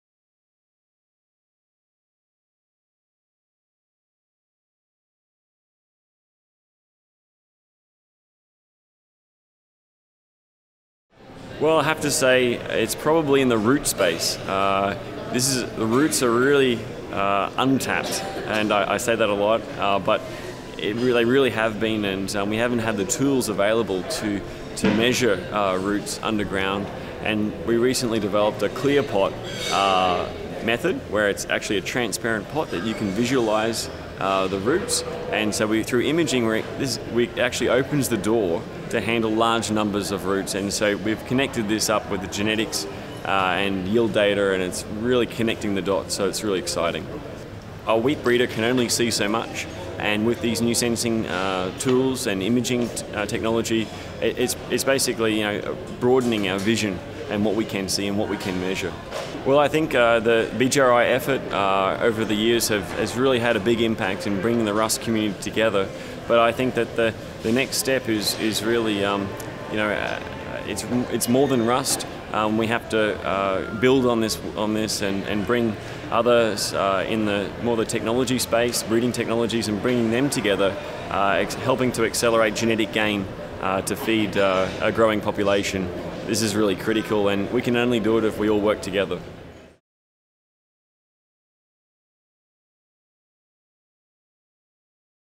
BGRI/IWC 2015 - Lee Hickey: The Future of Wheat Research?
We asked a number of wheat scientists from around the world the same question: What's the most important thing happening in wheat research today? And what is the future of wheat research?

Filmed at the 2015 Borlaug Global Rust Initiative Technical Workshop & International Wheat Conference, Sydney, Australia

Produced by Chris Knight (Cornell University, USA) and Anika Miller (University of Queensland, Australia) for the Borlaug Global Rust Initiative and Hickey Lab, University of Queensland, Australia.